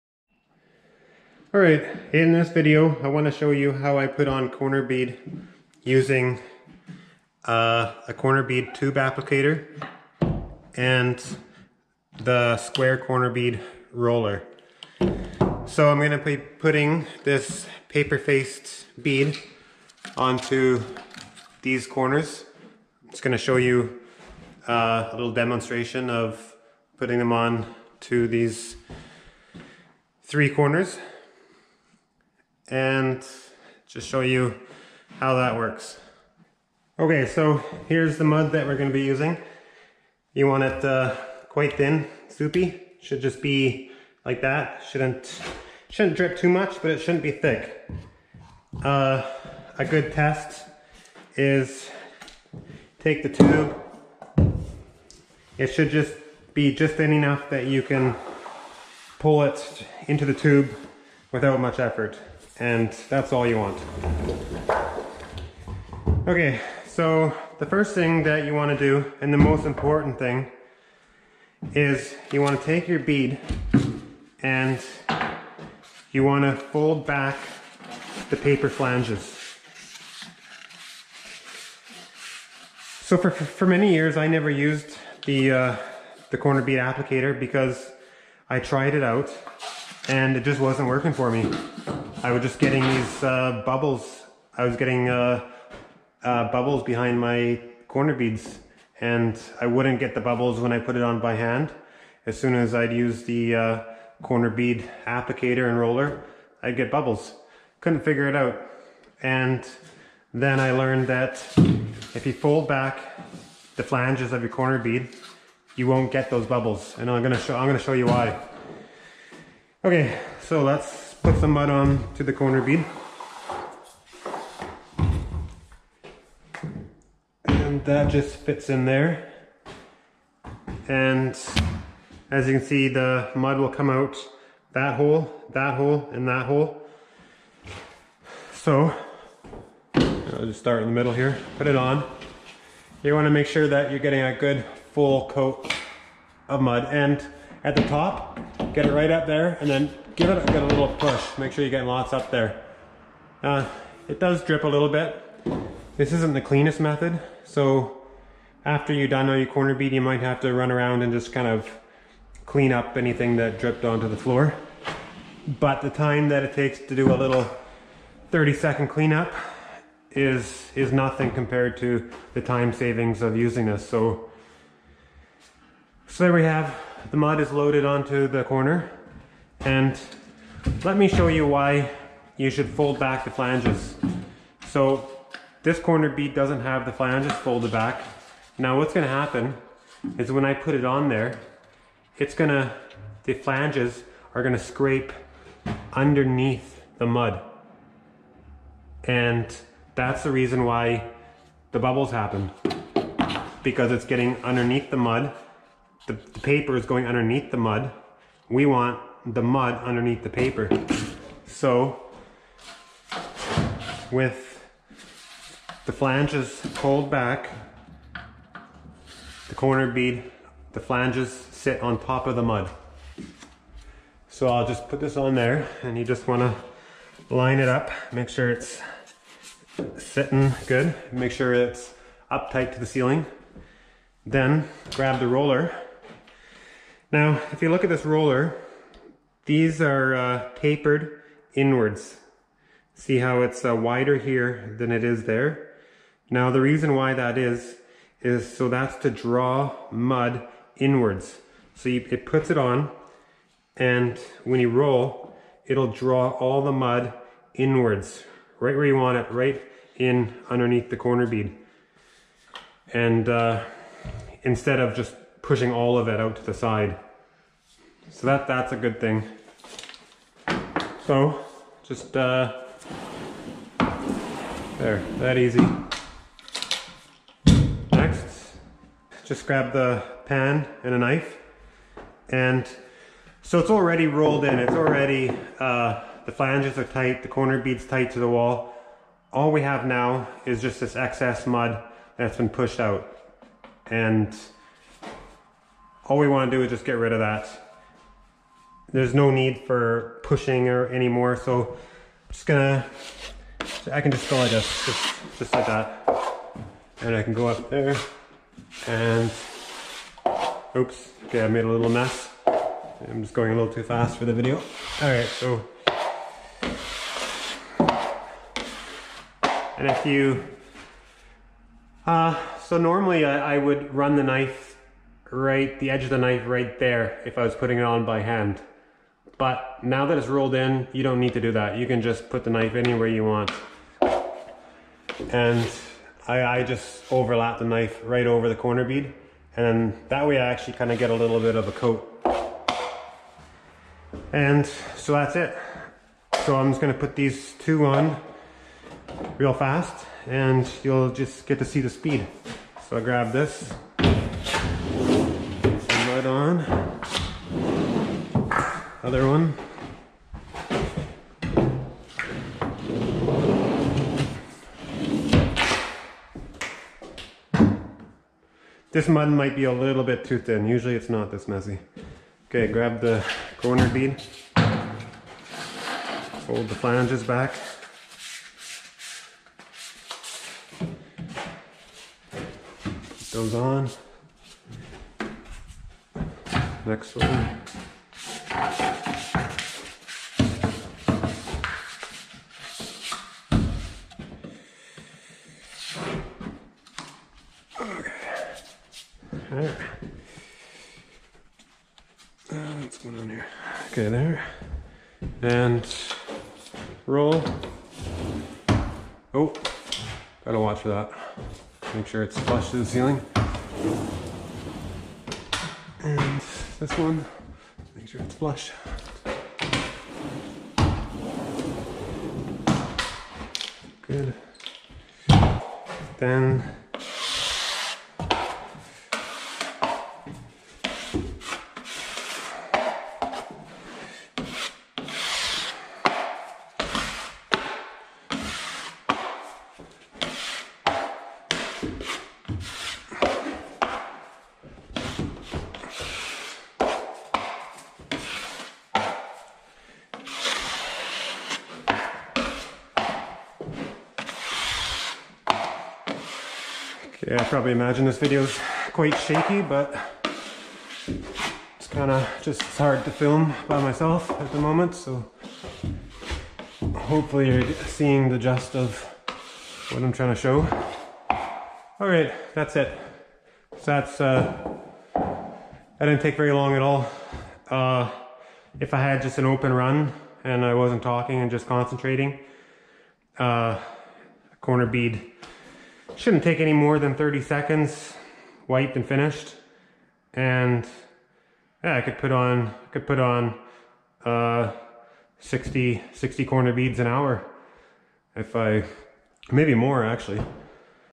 How to use the Drywall Cornerbead Applicator and Roller
How to use the Cornerbead applicator and Roller for installing Cornerbead on Drywall. Helpful tools for professional drywall taping and finishing.